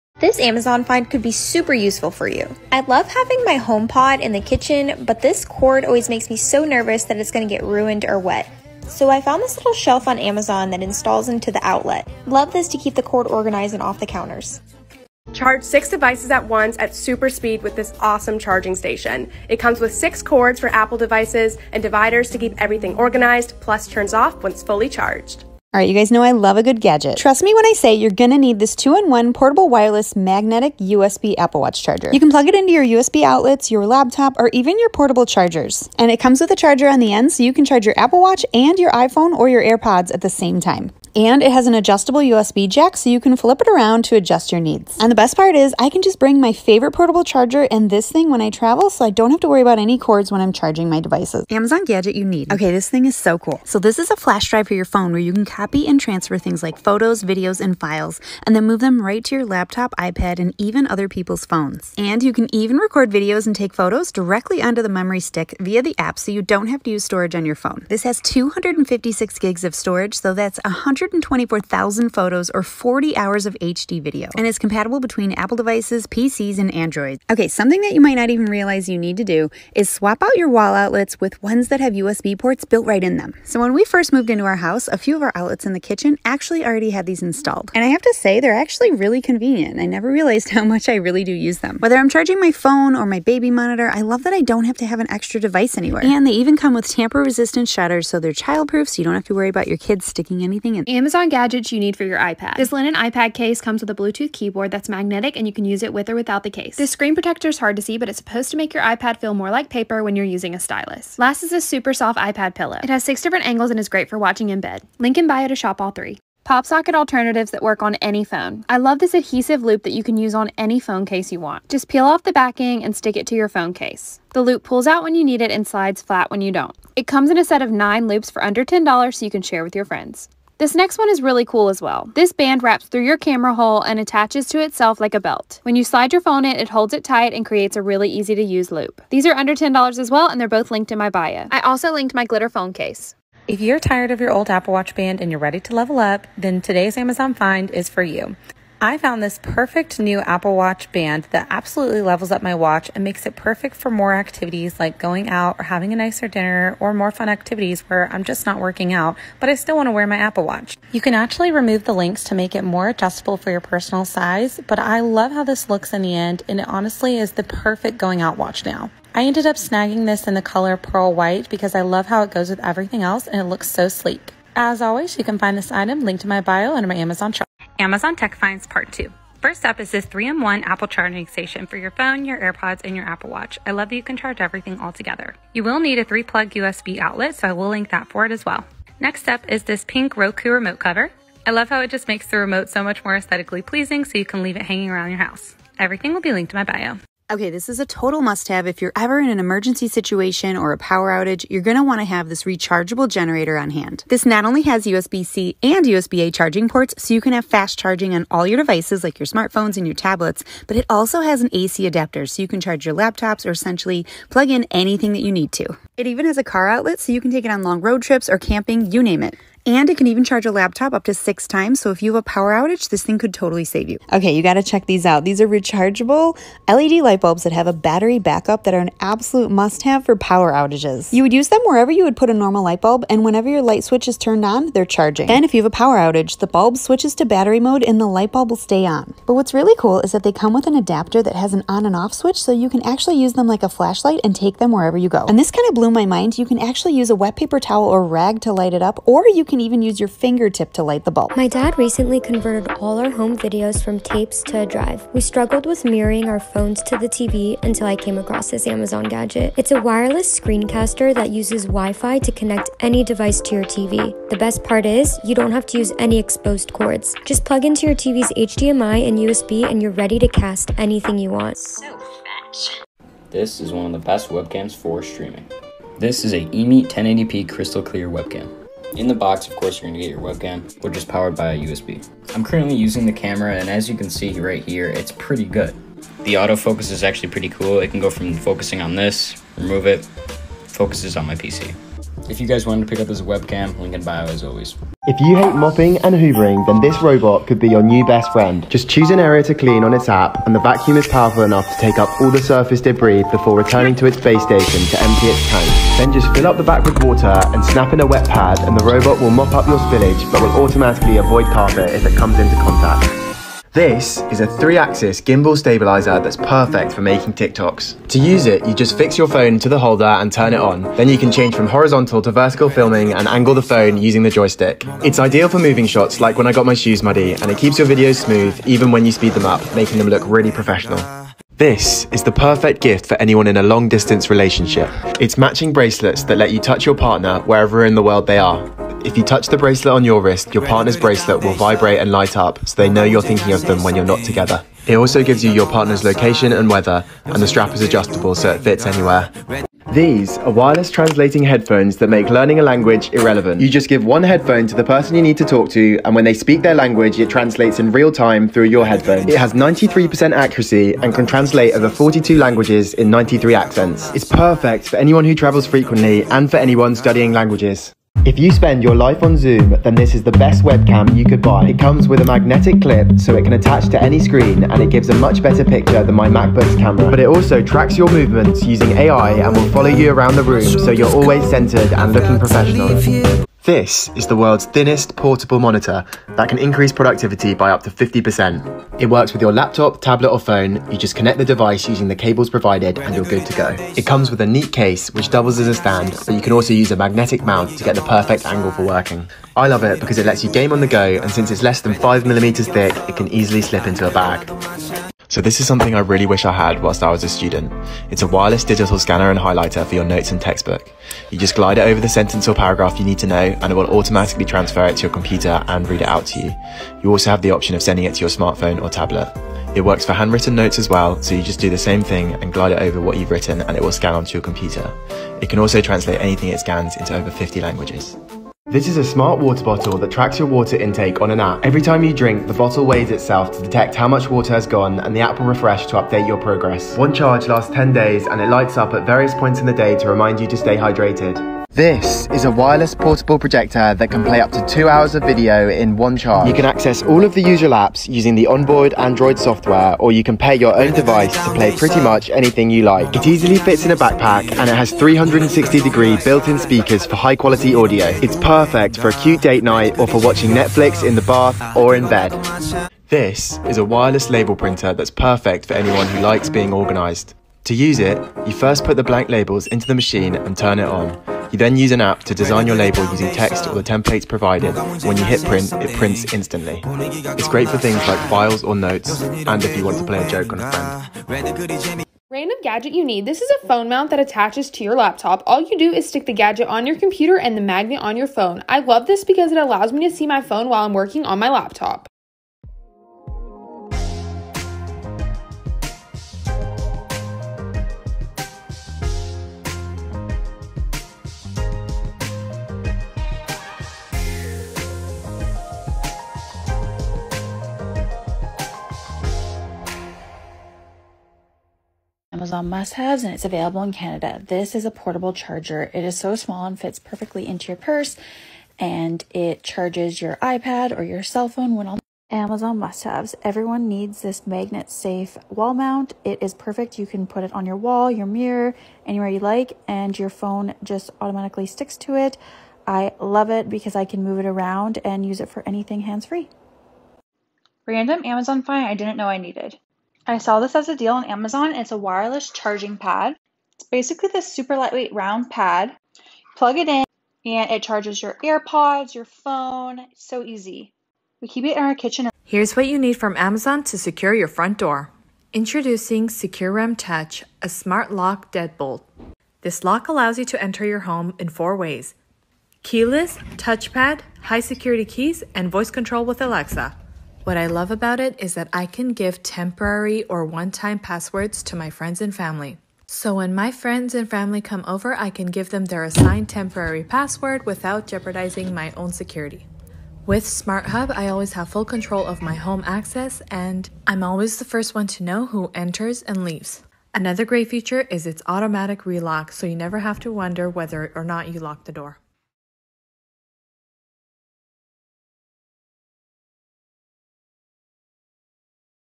TIKTOK AMAZON MUST HAVES | PT11
The videos used are not mine. TikTok usernames are below if you want to check out more from the creators! Have a beautiful day!

kaylargyle
Outlet shelf

orcashopco 00:15
Charging station

lindsayroggenbuck 00:26
Apple watch charger

lindsayroggenbuck 00:51
Flash drive for phone

lindsayroggenbuck 01:17
Smart outlet

kassyrandazzo 1:40
iPad case
Screen protector
iPad Pillow

kassyrandazzo 1:56
Phone grip band

lifewithlaurenheath 2:22
Apple watch band

lifewithlaurenheath 2:56
Charging station
3 plug outlet
Roku remote cover

lindsayroggenbuck 3:16

lindsayroggenbuck 3:43
Smart light bulbs

add2bag 4:10
Screen caster

blizzardtech 4:33

thejunglebadger 4:55
Robot Vacuum

thejunglebadger 5:13

thejunglebadger 5:31
Bond touch

thejunglebadger 5:49
Translator Earbuds

thejunglebadger 6:09

thejunglebadger 6:23
Portable monitor

thejunglebadger 6:45
Scanmarker Air

thejunglebadger 7:09
Smart water bottle

thejunglebadger 7:21
Portable projector

thejunglebadger 7:43
Label maker

kortneyandkarlee 8:02
Laptop mount

mash.elle 8:12
Memory card holder
Phone stand
Slim portable charger
Blue light blocking glasses
USB-C Hub

chaltner 8:32
Portable charger

chaltner 8:42
Magnetic phone mount

audreymadstowe 8:57
Wireless charger

sadiisohrab 9:12
Smart lock